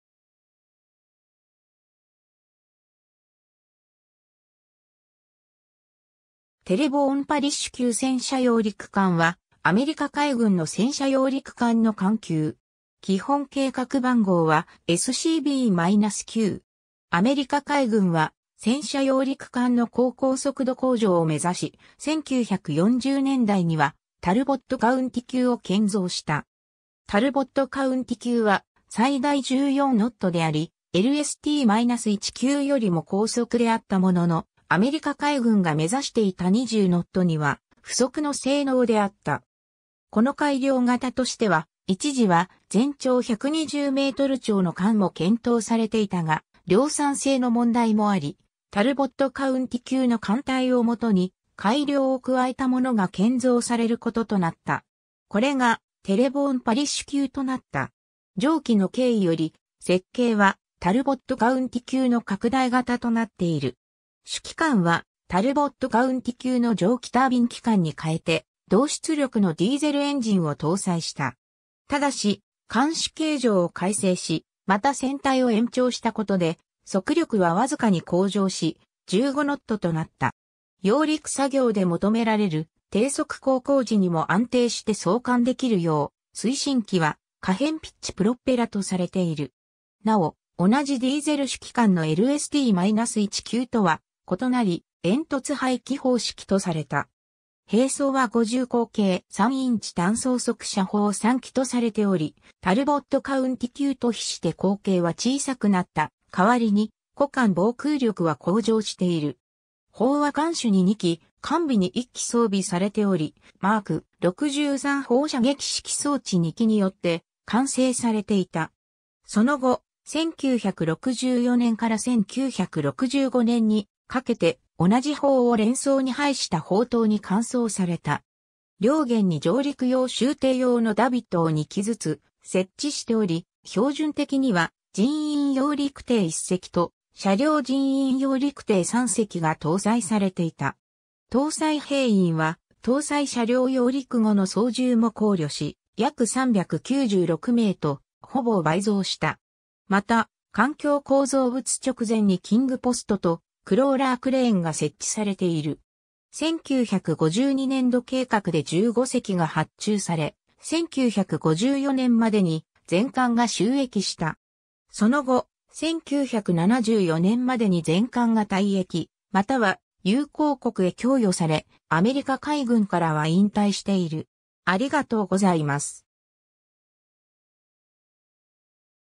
テレボーン・パリッシュ級戦車揚陸艦
テレボーン・パリッシュ級戦車揚陸艦（英語: Terrebonne Parish-class tank landing ship）はアメリカ海軍の戦車揚陸艦の艦級。
基本計画番号はSCB-9。
 アメリカ海軍は、戦車揚陸艦の航行速度向上を目指し、1940年代にはタルボット・カウンティ級を建造した。
タルボット・カウンティ級は最大14ノットであり、LST-1級よりも高速であったものの、アメリカ海軍が目指していた20ノットには不足の性能であった。
この改良型としては、一時は全長120m超の艦も検討されていたが、量産性の問題もあり、タルボット・カウンティ級の艦体を基に、改良を加えたものが建造されることとなった。
これが、テレボーン・パリッシュ級となった。
 上記の経緯より、設計はタルボット・カウンティ級の拡大型となっている。
主機関は、タルボット・カウンティ級の蒸気タービン機関にかえて、同出力（6,000馬力）のディーゼルエンジンを搭載した。
ただし艦首形状を改正し、また船体を延長したことで速力はわずかに向上し、15ノットとなった。
揚陸作業で求められる低速航行時にも安定して操艦できるよう、推進器は可変ピッチ・プロペラ（CPP）とされている。
なお、同じディーゼル主機関のLST-1級とは異なり、煙突排気方式とされた。
 兵装は50口径3インチ単装速射砲3基とされており、タルボット・カウンティ級と比して口径は小さくなったかわりに個艦防空力は向上している。
砲は艦首に2基、艦尾に1基装備されており、Mk.63 砲射撃指揮装置2基によって管制されていた。
その後、1964年から1965年にかけて、同じ砲を連装に配した砲塔に換装された。
 両舷に上陸用舟艇用のダビットを2基ずつ設置しており、標準的には人員揚陸艇（LCPL）1隻と車両人員揚陸艇（LCVP）3隻が搭載されていた。
搭載兵員は、搭載車両揚陸後の操縦も考慮し、約396名とほぼ倍増した。
また艦橋構造物直前にキング・ポストとクローラー・クレーンが設置されている。
 1952年度計画で15隻が発注され、1954年までに全艦が就役した。
その後、1974年までに全艦が退役又は友好国へ供与され、アメリカ海軍からは引退している。